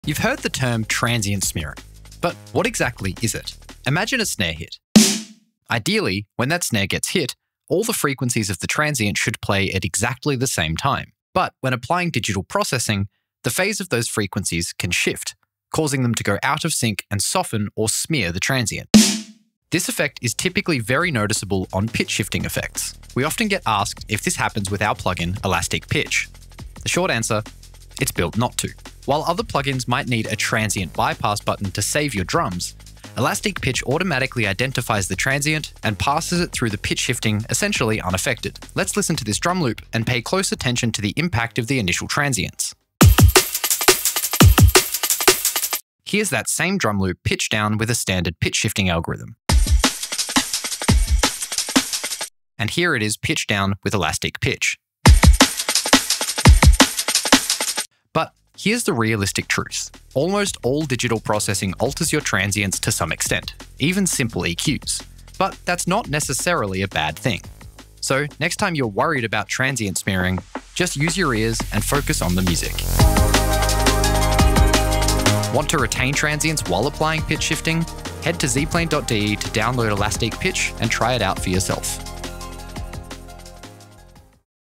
What is Transient Smearing in Audio?
You might have heard the term transient smearing, but what is it? What causes it, and does it really matter?